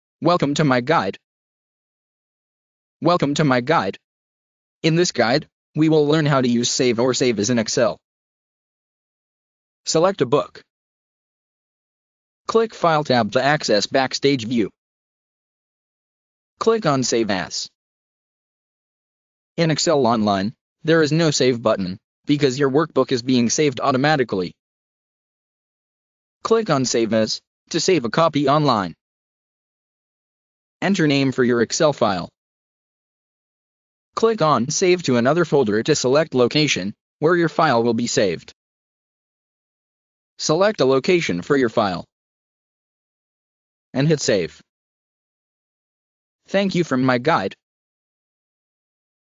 MyGuide: Using Save or Save As in Excel
1. Welcome to MyGuide
In this Guide, we will learn how to use Save or Save As in Excel.

2. Select a book

3. Click "File" tab to access Backstage view

4. Click on "Save As"

5. In Excel Online, there is no save button because your workbook is being saved automatically

6. Click on "Save As", to save a copy online

7. Enter name for your excel file

8. Click on "Save to another folder"
To look for a location where your file will be saved

9. Select a location for your file

10. And hit "Save"!